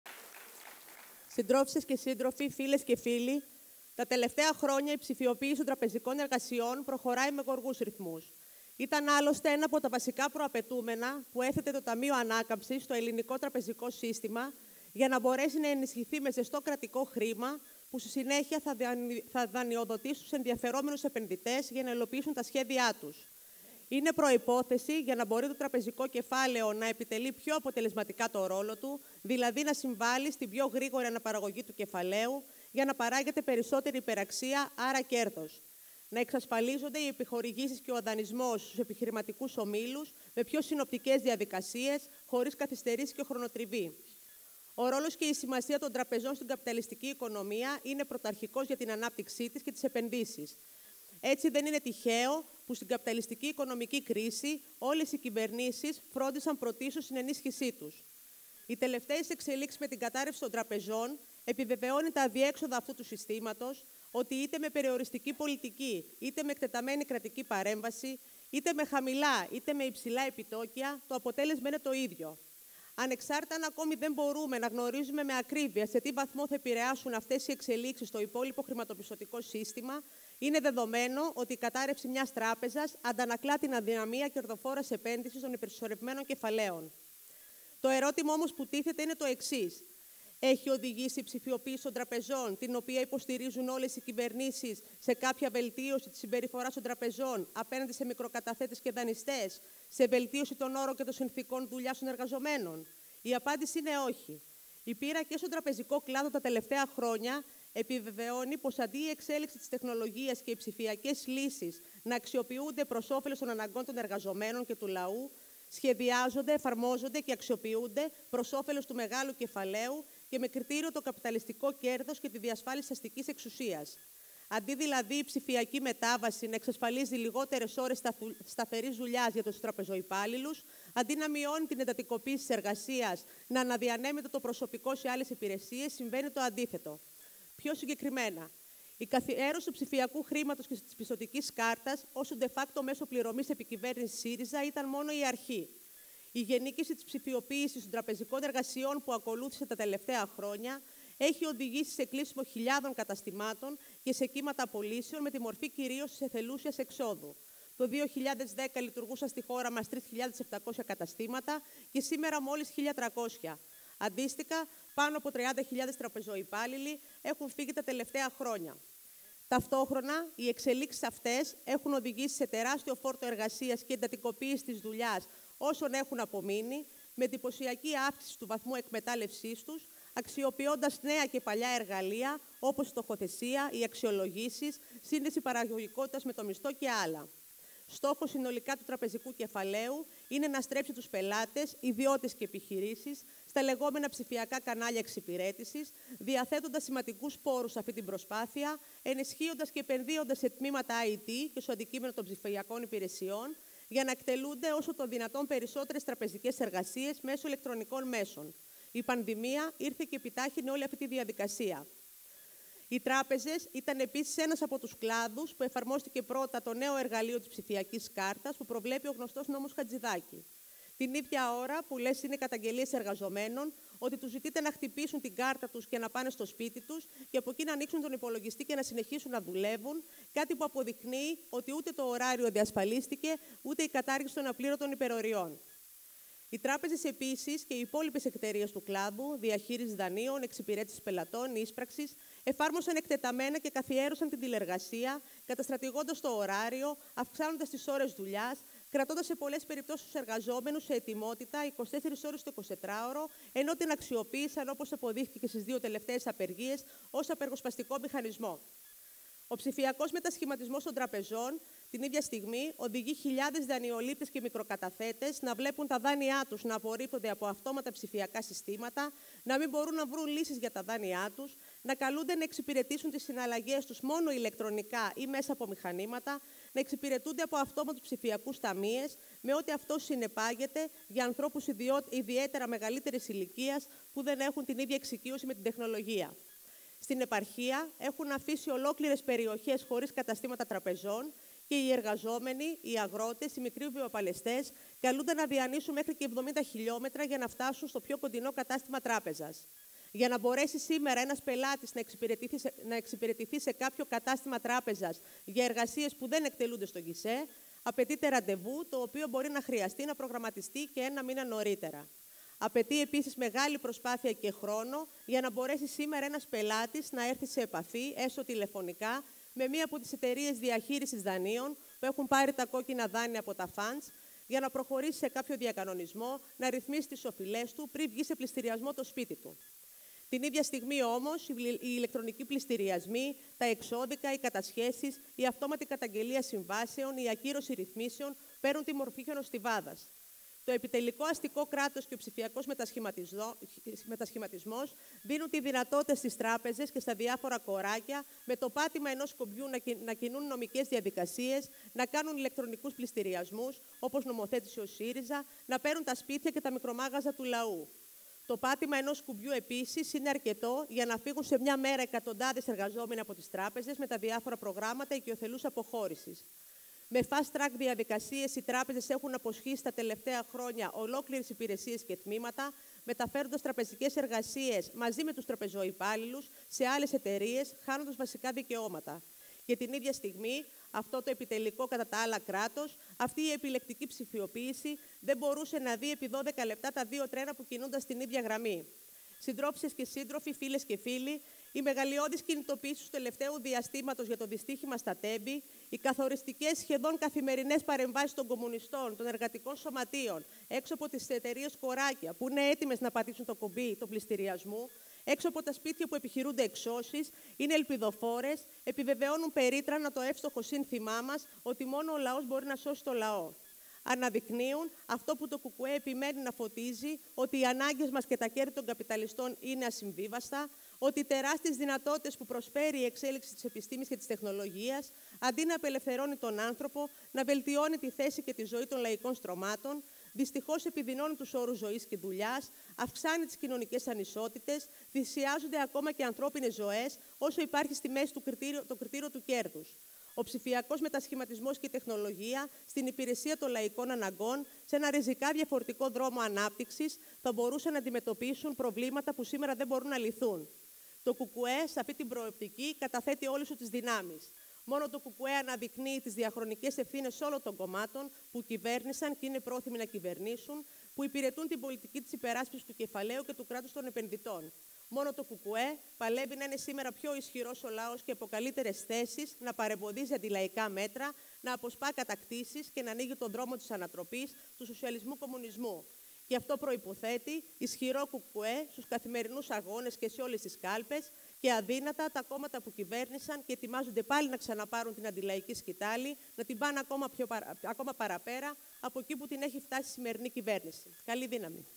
Παρέμβαση της Ε. Μπεκιάρη στην εκδήλωση του ΚΚΕ για τον «ψηφιακό μετασχηματισμό»
Κατάμεστο ήταν το πρωί της Κυριακής το Αμφιθέατρο «Μιλτιάδης Έβερτ», στην Τεχνόπολη στο Γκάζι, όπου πραγματοποιήθηκε η εκδήλωση του ΚΚΕ με θέμα «Ψηφιακός μετασχηματισμός για τη ζωή και τις ανάγκες των πολλών ή για τα κέρδη των λίγων - Μύθοι και πραγματικότητα».

Διοργανωτές ήταν οι Τομεακές Οργανώσεις Χρηματοοικονομικών και Λογιστικών Υπηρεσιών, Τηλεπικοινωνιών, Τοπικής - Περιφερειακής και Κεντρικής Διοίκησης της ΚΟ Αττικής του ΚΚΕ

Την κεντρική ομιλία έκανε ο Μάκης Παπαδόπουλος, μέλος του ΠΓ της ΚΕ του ΚΚΕ και υποψήφιος βουλευτής Επικρατείας.

Ακολούθησαν παρεμβάσεις από τις Ελπίδα Στεφανίδη (Νάvτια), υποψήφια βουλευτή τoυ ΚΚΕ στον Νότιο Τομέα, εργαζόμενη σε εταιρεία πληροφορικής και μέλος του ΣΕΤΗΠ, Ειρήνη Μπεκιάρη, υποψήφια βουλευτή του ΚΚΕ στον Δυτικό Τομέα, γραμματέα της ΤΟ Χρηματοικονομικών & Λογιστικών Υπηρεσιών της ΚΟΑ, Βέτα Πανουτσάκου, υποψήφια βουλευτή του ΚΚΕ στην Α' Αθήνας, μέλος της ΕΕ τnς ΑΔΕΔΥ, πρόεδρο του Συλλόγου Υπαλλήλων Περιφέρειας Αττικής, Ολγα Κολιούση, μέλος του Τμήματος Οικονομίας τnς ΚΕ του ΚΚΕ, Αλεξάνδρα Kαπαρού, πρόεδρο της Ένωσης Λογιστών Ελεγκτών Περιφέρειας Αττικής.

Στην παρέμβασή της η Ειρήνη Μπεκιάρη, υποψήφια βουλευτής του ΚΚΕ στον Δυτικό Τομέα, γραμματέας της ΤΟ Χρηματοικονομικών & Λογιστικών Υπηρεσιών της ΚΟΑ, η οποία μεταξύ άλλων υπογράμμισε «Τα τελευταία χρόνια η ψηφιοποίηση των τραπεζικών εργασιών προχωράει με γοργούς ρυθμούς. Ήταν άλλωστε ένα από τα βασικά προαπαιτούμενα που έθετε το Ταμείο Ανάκαμψης στο ελληνικό τραπεζικό σύστημα για να μπορέσει να ενισχυθεί με ζεστό κρατικό χρήμα που στη συνέχεια θα δανειοδοτήσει τους ενδιαφερόμενους επενδυτές για να υλοποιήσουν τα σχέδιά τους.

Είναι προϋπόθεση για να μπορεί το τραπεζικό κεφάλαιο να επιτελεί πιο αποτελεσματικά το ρόλο του, δηλαδή να συμβάλει στην πιο γρήγορη αναπαραγωγή του κεφαλαίου, για να παράγεται περισσότερη υπεραξία, άρα κέρδος. Να εξασφαλίζονται οι επιχορηγήσεις και ο δανεισμός στους επιχειρηματικούς ομίλους με πιο συνοπτικές διαδικασίες, χωρίς καθυστερήσεις και χρονοτριβή. (...)

Το ερώτημα όμως που τίθεται είναι το εξής; Έχει οδηγήσει η ψηφιοποίηση των τραπεζών, την οποία υποστηρίζουν όλες οι κυβερνήσεις, σε κάποια βελτίωση της συμπεριφοράς των τραπεζών απέναντι σε μικροκαταθέτες και δανειστές, σε βελτίωση των όρων και συνθηκών εργασίας των εργαζομένων;

Η απάντηση είναι ΟΧΙ.

Η πείρα και στον τραπεζικό κλάδο τα τελευταία χρόνια επιβεβαιώνει πως αντί η εξέλιξη της τεχνολογίας και οι ψηφιακές λύσεις να αξιοποιούνται προς όφελος των αναγκών των εργαζομένων και του λαού, σχεδιάζονται, εφαρμόζονται και αξιοποιούνται προς όφελος του μεγάλου κεφαλαίου και με κριτήριο το καπιταλιστικό κέρδος και την διασφάλιση της αστικής εξουσίας».